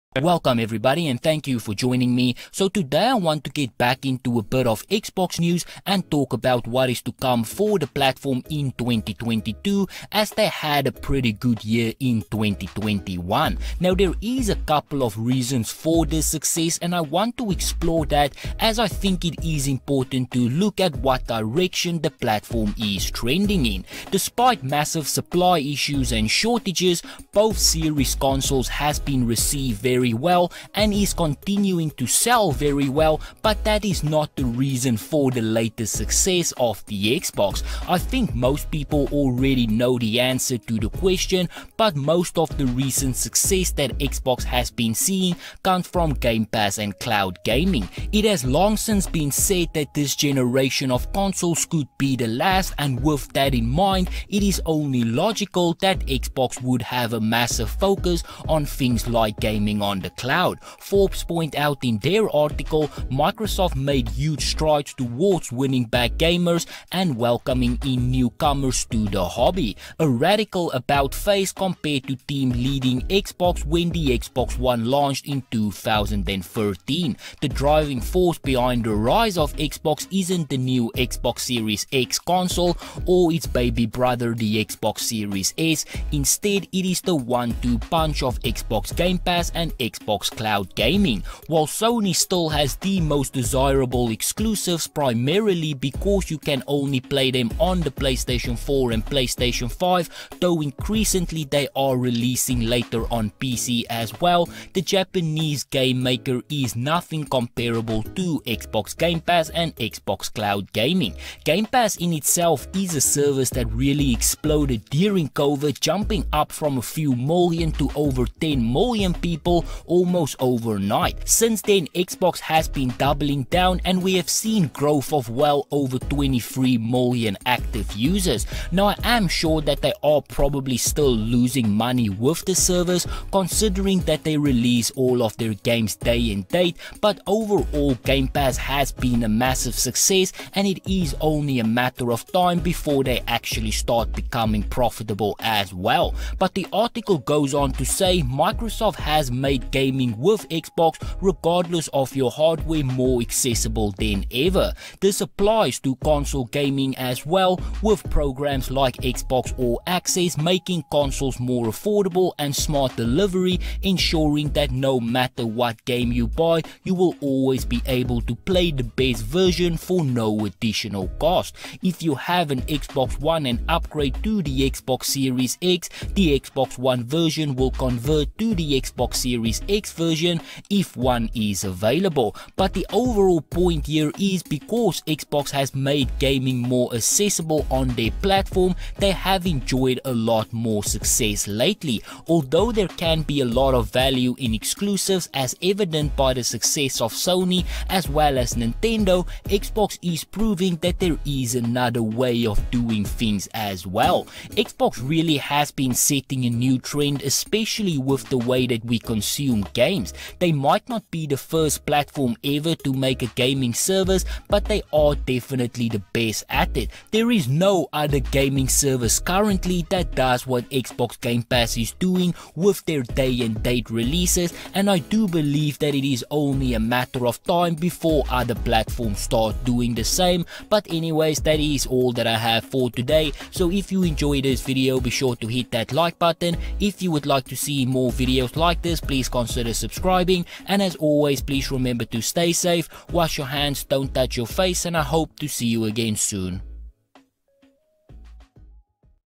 Xbox Success In 2021 | Xbox Series X Had A Good Year | Future Of Xbox Game Pass
Xbox is having a good year. Although the Xbox Series X and the Xbox Series S has been in short supply, both consoles have been doing really well and despite everything, The Xbox Series S has been one of the best selling consoles of November.

Sources